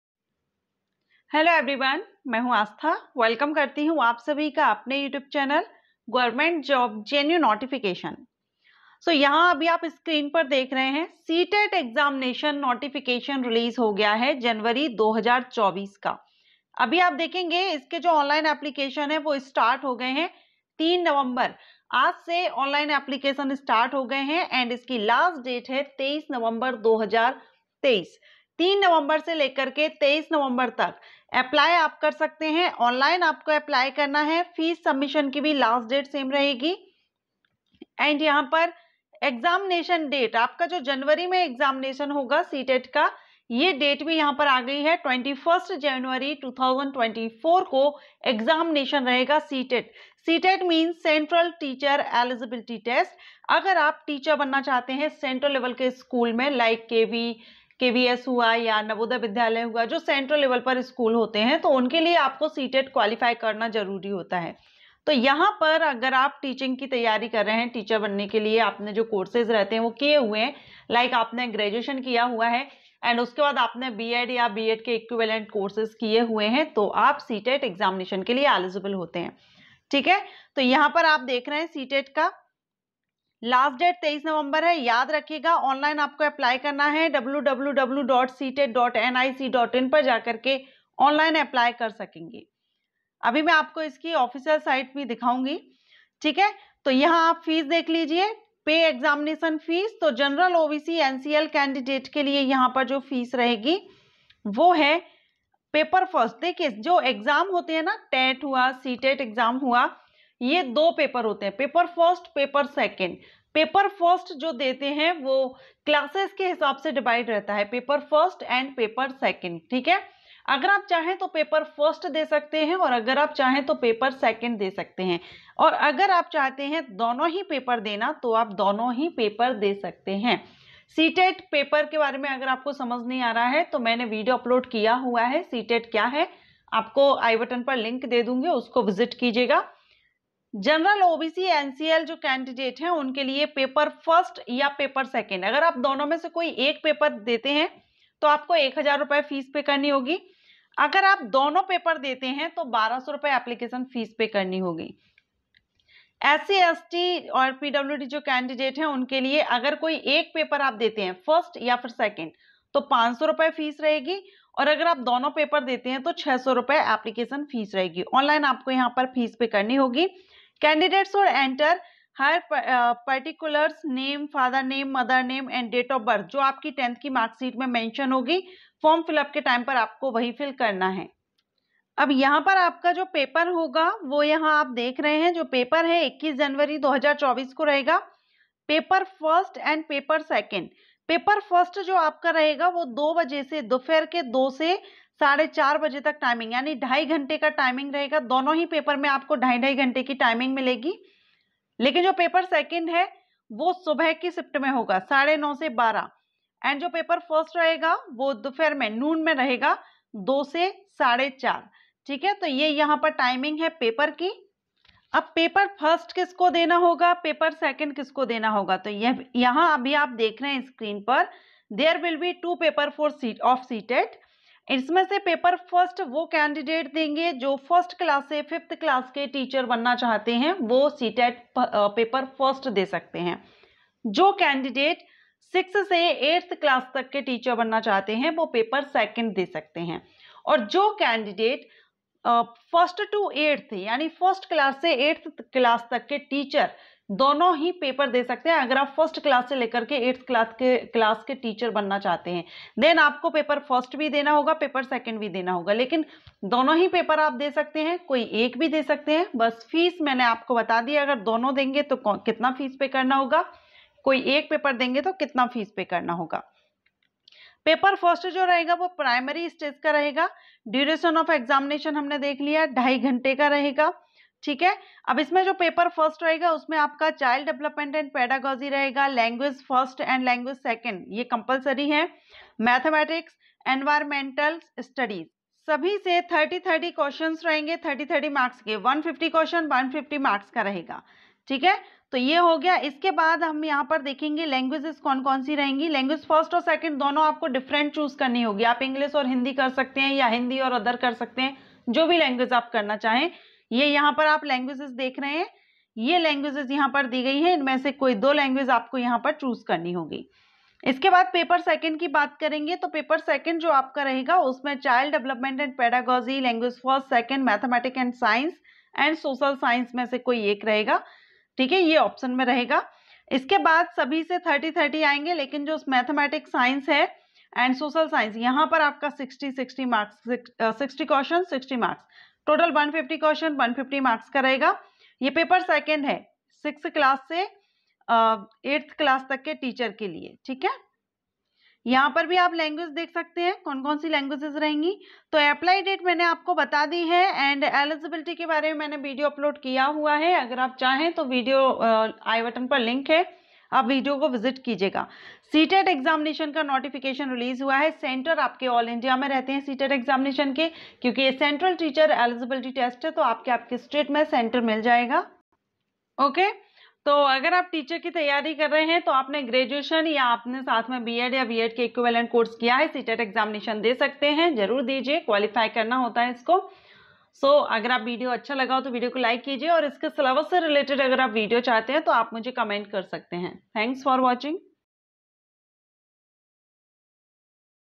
CTET 2024 notification Out | CTET 2024 application form | exam date
Friends, video main CTET 2024 notification detail and exam date ki jaankari di gayi hai,

CTET 2024
CTET online apply 2024
CTET Full form
CTET notification kab aayega
CTET 2024 application form

Link